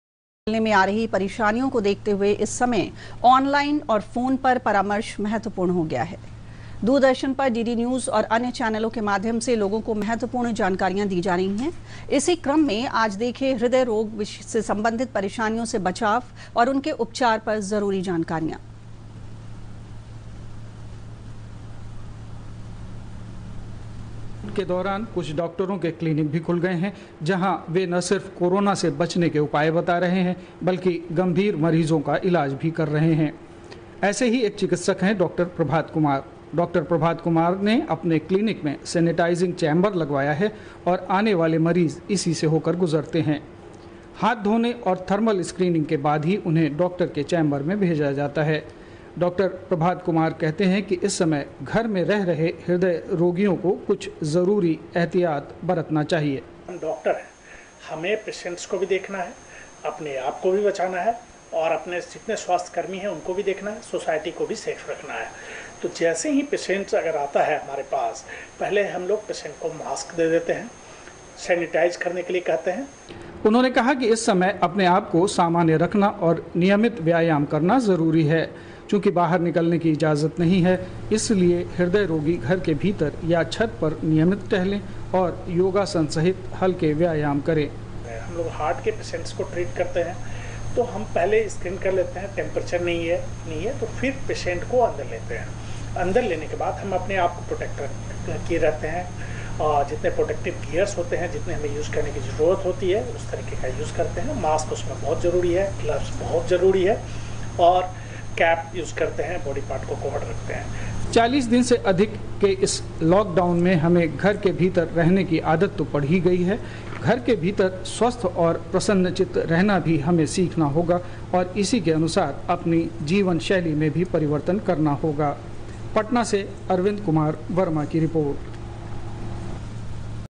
Covid-19: घर में रहते हुए सेहत का रखें ध्यान.
पटना के प्रख्यात हृदयरोग विशेषज्ञ डॉ प्रभात कुमार बता रहे हैं, लॉकडाउन के दौरान घर में रहते हुए कैसे रखें अपने दिल का खयाल.